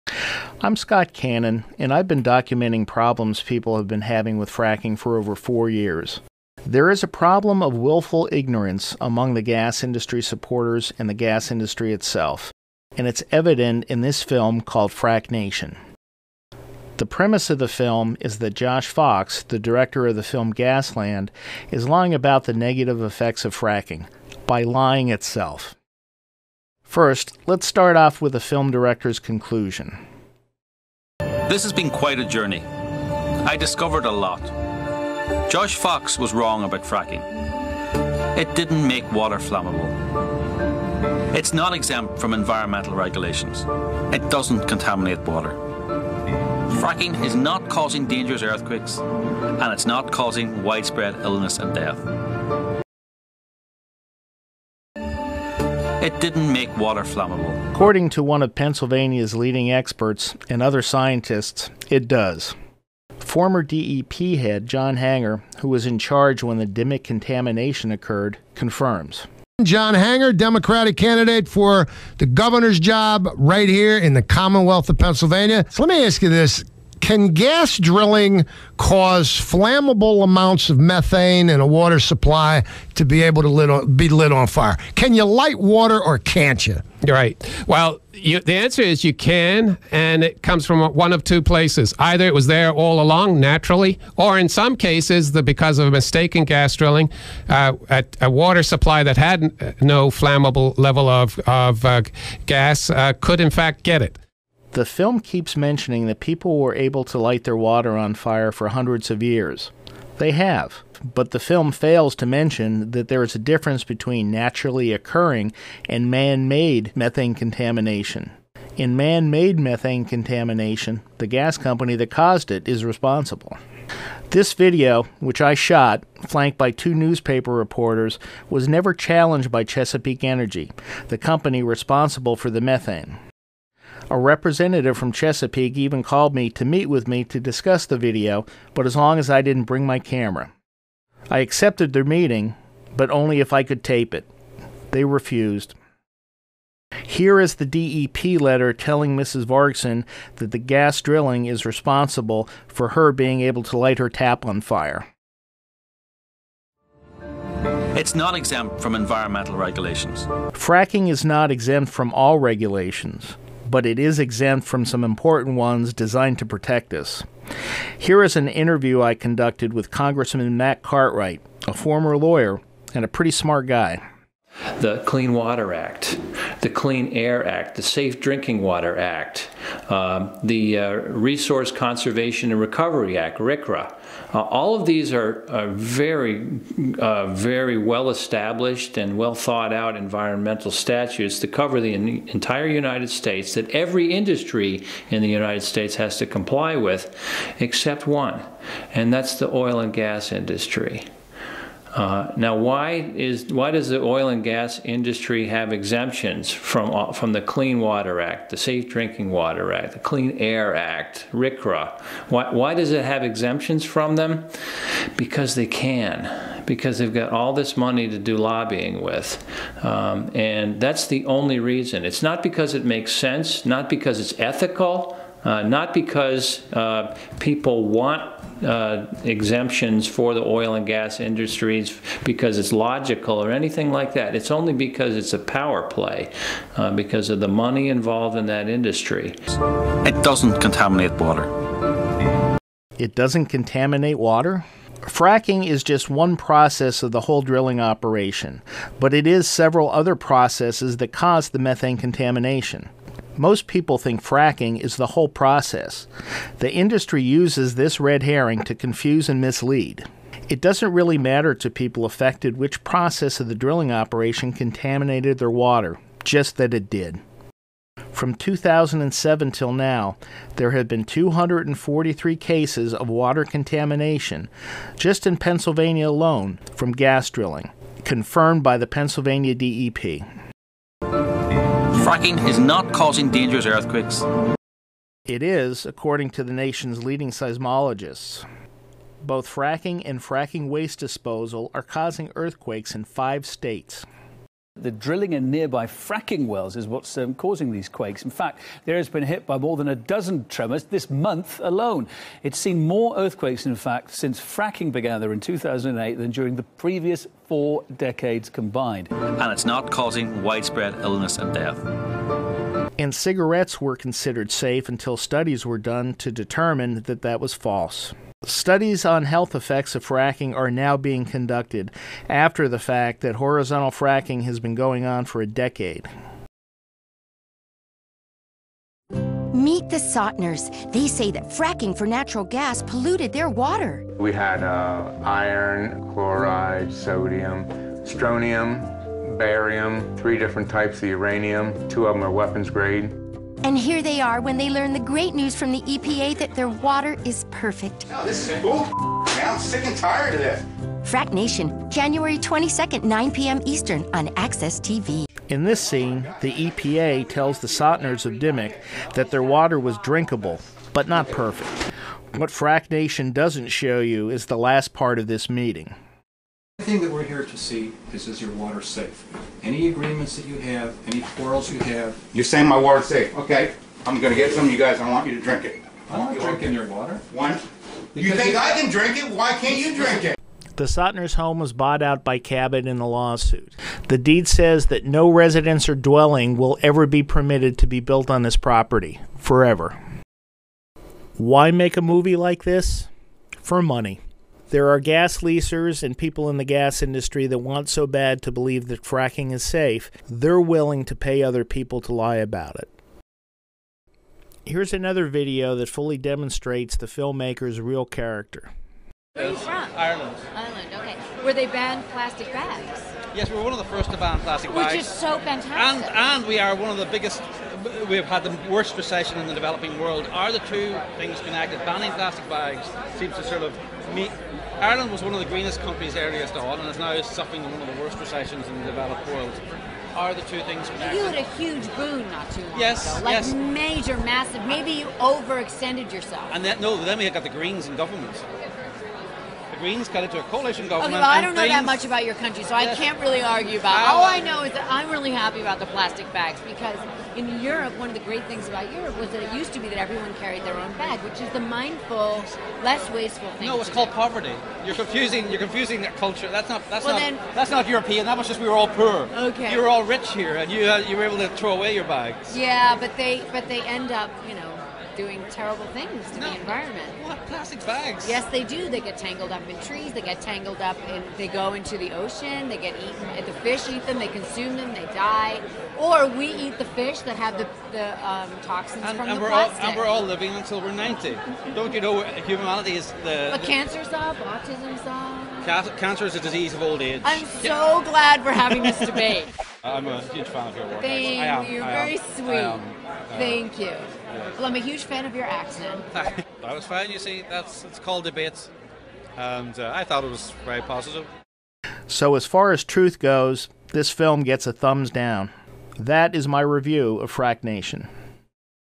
Fracknation Review
by Scott Cannon I take a look at the way the film Fracknation bends the truth in relation to fracking or hydraulic fracturing. The entire list of Pennsylvania water contaminations from fracking.